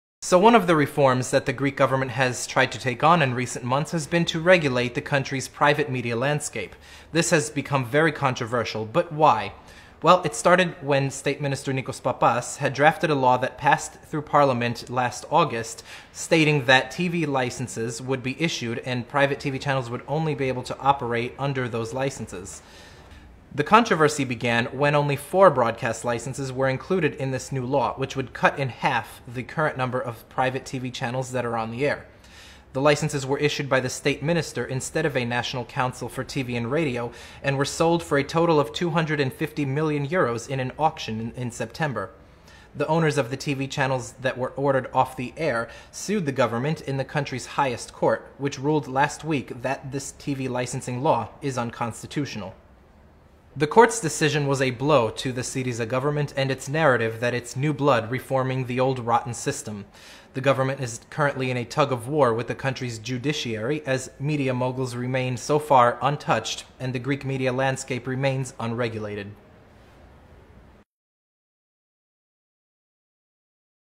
The Greek TV Licencing Series Story Explained
Whether it's a parody, comedy, or drama, the Greek TV licencing series has triggered a major debate over corruption and control of the media.

Watch our video with Nick Barnets explaining the story.